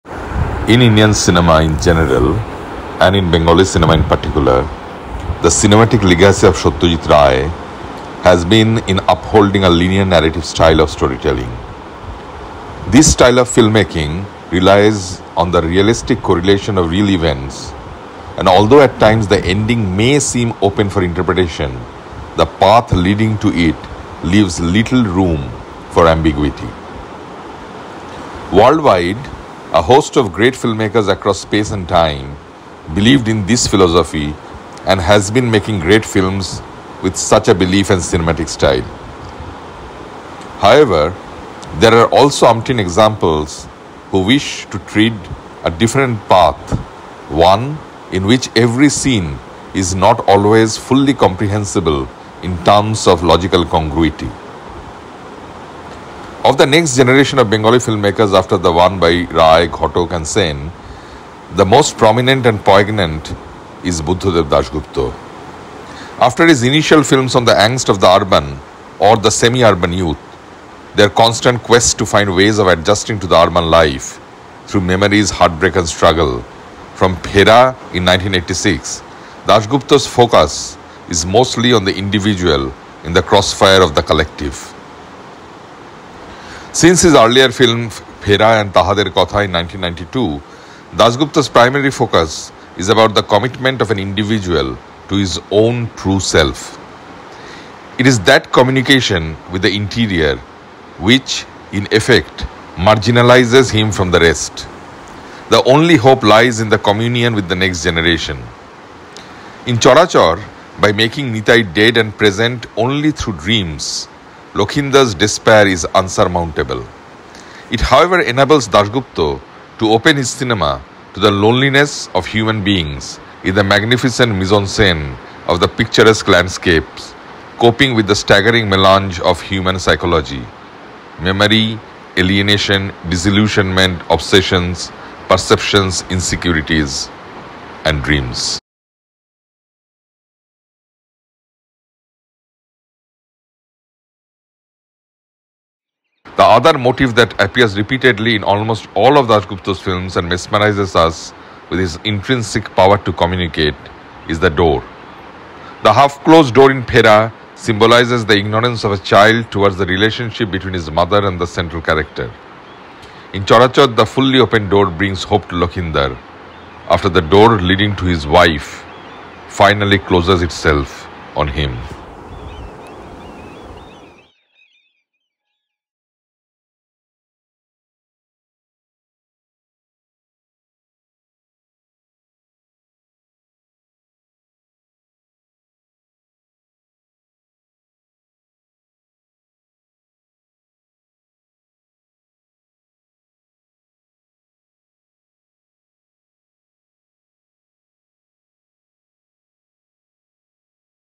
Buddhadeb Dasgupta's Charachar | Films, Techniques, Motifs | Amitava Nag
A poet himself, Buddhadeb Dasgupta's films were often termed poetic for their long takes and incorporation of symbols and dreams.

As a tribute to Dasgupta's on his death anniversary (10 June), Amitava Nag reads excerpts from the chapter 'Charachar' from his '75 years 75 films: India's Cinematic Journey'.

Amitava mentions Dasgupta's films that explored the angst of the urban youth, the commitment of the individual to his own true self, his techniques and recurrent motifs such as the door.